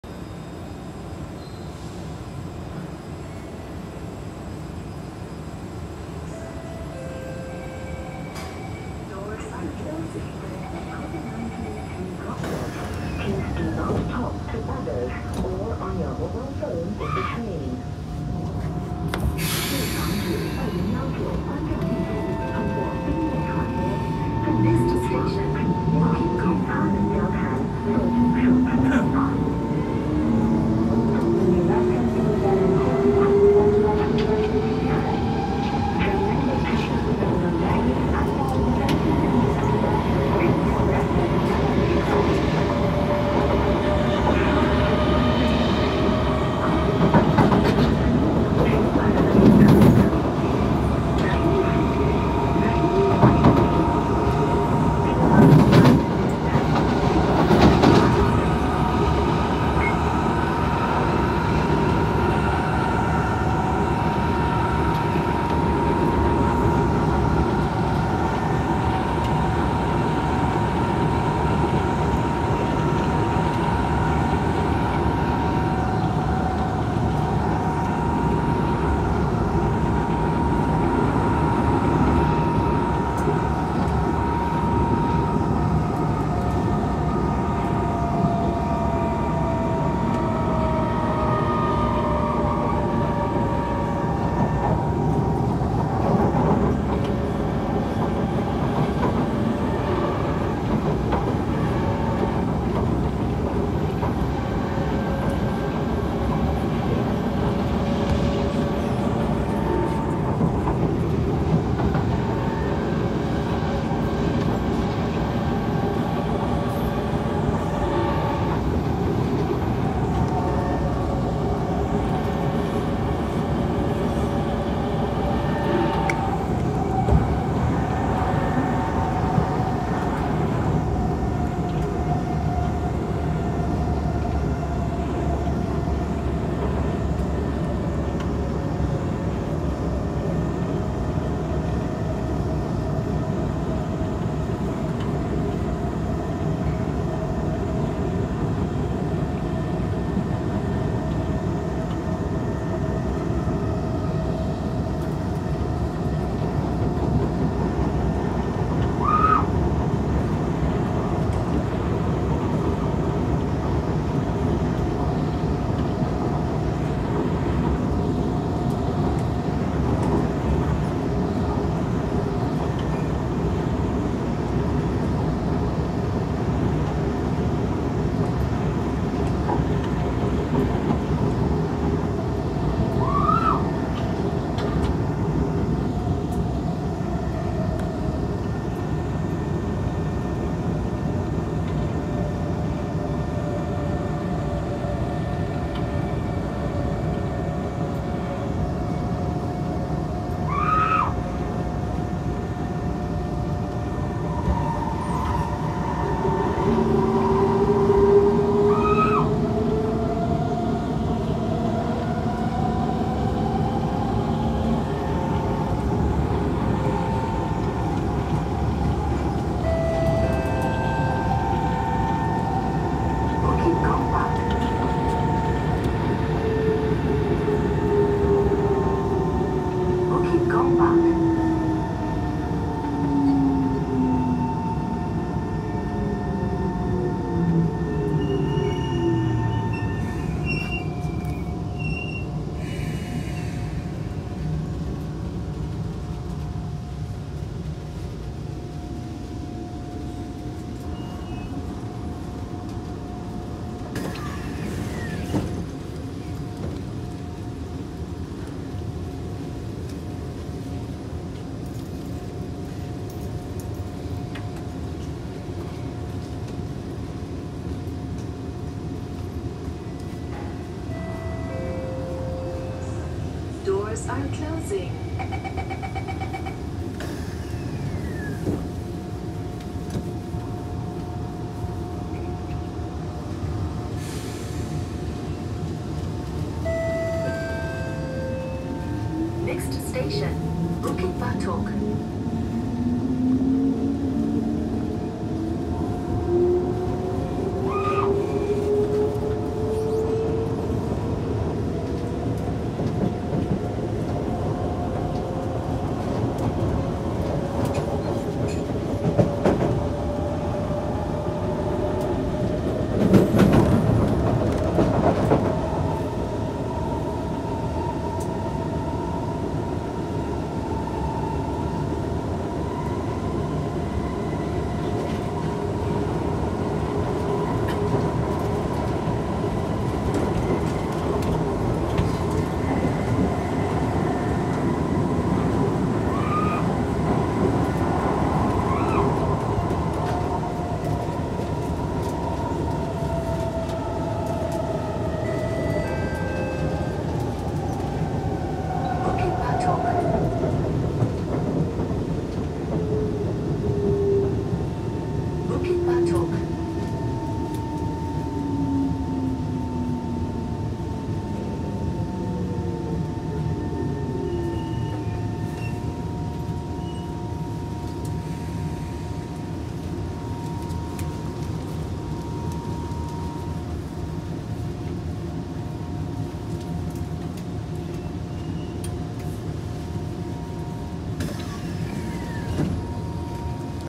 ⁴ᴷ⁶⁰ [Morning Scream] SMRT Trains, NSL Train Ride [CCK → Bt Batok] - Kawasaki C151 105/106
Date of Recording: 23 April 2021
Slot: 402
Line to Destination: North South Line to Jurong East A
Rolling Stock: Kawasaki Heavy Industries & Kinki Sharyo - C151 105/106
Stretch: NS4 BP1 Choa Chu Kang to NS2 Bukit Batok

Ok. This one is next. Back to NSL after weeks serving EWL. I like how the motor thrash works especially there’s one screaming inside the train every single day and in the morning as well.